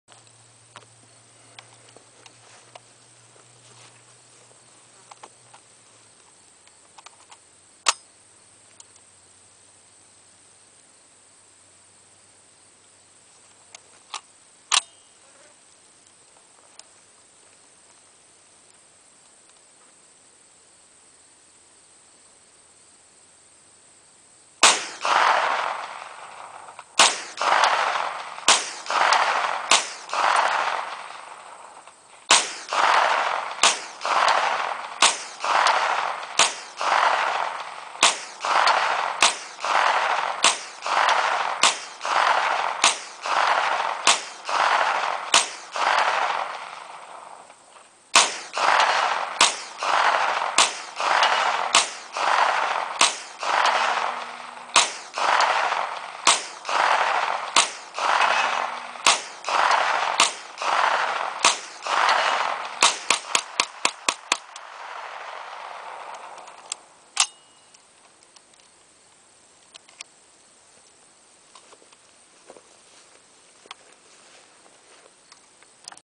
Uzi 9mm Vector Arms
Me shooting my Vector arms Uzi 9mm.
The ping you hear is a metal target over 100 yards away, size of a chest